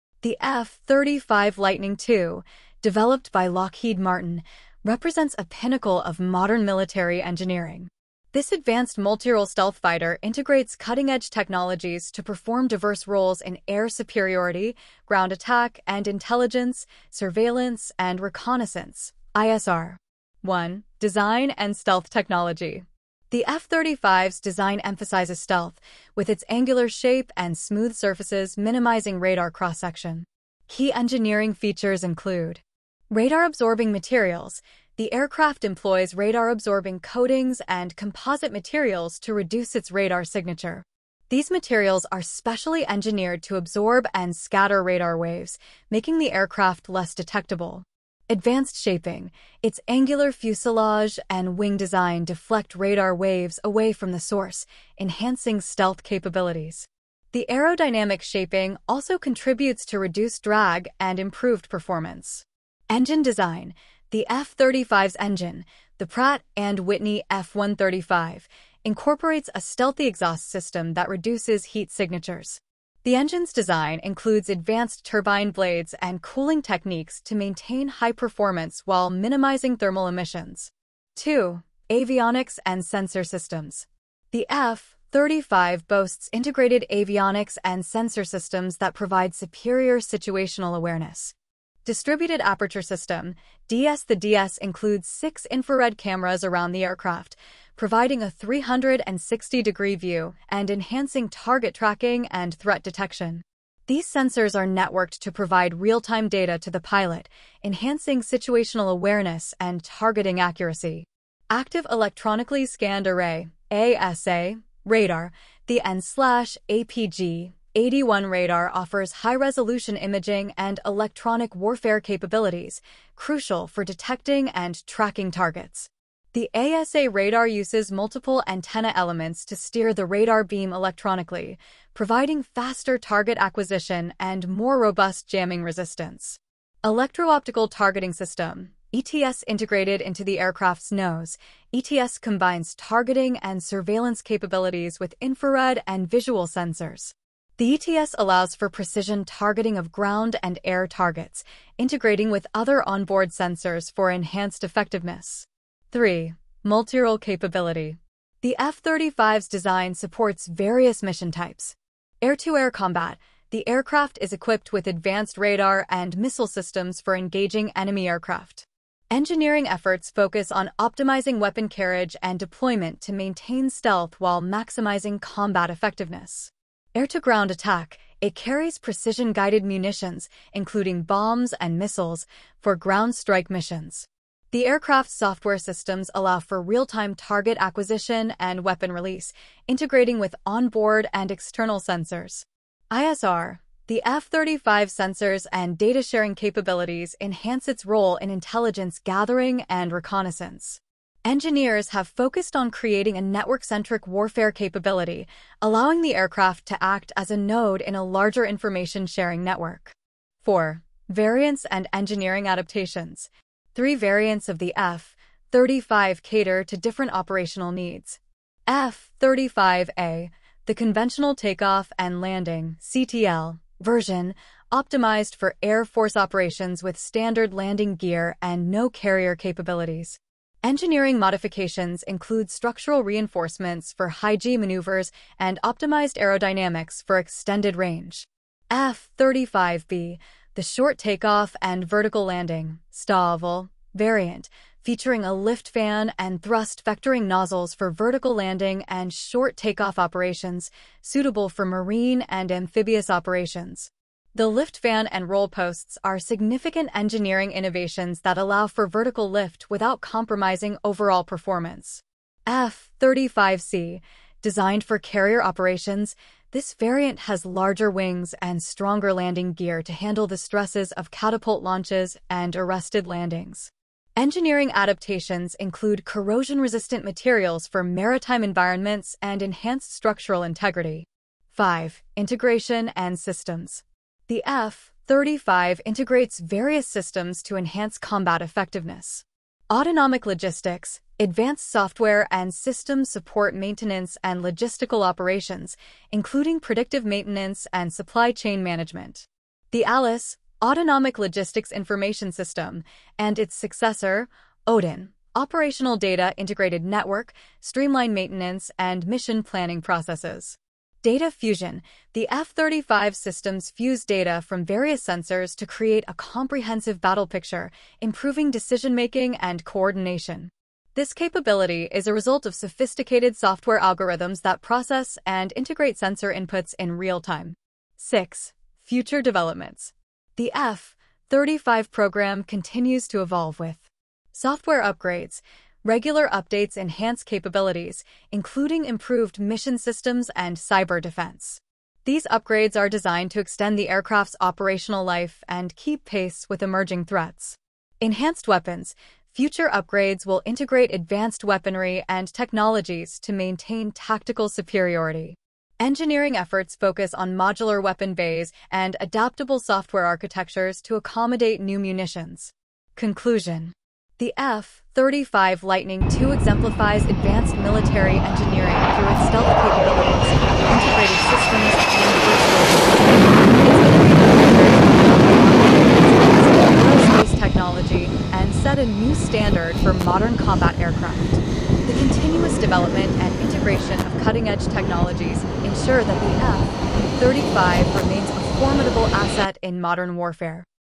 Engineering Insights into the F 35 Lightning II | Military Engineering
Discover the engineering marvel behind the F-35 Lightning II, an advanced multirole stealth fighter developed by Lockheed Martin. In this video, we delve into the cutting-edge technologies that make the F-35 a game-changer in modern military aviation. From its stealth design and radar-absorbing materials to its integrated avionics and sensor systems, learn how the F-35 maintains air superiority and excels in ground attack and ISR missions.

🔹 **Design and Stealth Technology**: Explore the F-35's radar-absorbing materials, advanced shaping techniques, and stealthy engine design that reduce its radar and heat signatures.

🔹 **Avionics and Sensor Systems**: Understand how the Distributed Aperture System (DAS), AESA radar, and Electro-Optical Targeting System (EOTS) provide superior situational awareness and targeting precision.

🔹 **Multirole Capability**: See how the F-35 performs in air-to-air combat, air-to-ground attacks, and intelligence, surveillance, and reconnaissance (ISR) roles.

🔹 **Variants and Engineering Adaptations**: Learn about the unique features of the F-35A, F-35B, and F-35C variants, each designed for specific operational needs.

🔹 **Integration and Systems**: Discover the advanced software and systems that support the F-35's maintenance, logistics, and data fusion capabilities.

🔹 **Future Developments**: Stay informed about ongoing software upgrades and new weapon integrations that keep the F-35 at the forefront of military technology.

Join us as we uncover the innovations that make the F-35 Lightning II an unparalleled force in modern combat.

Explore the history, science, and technology of military engineering.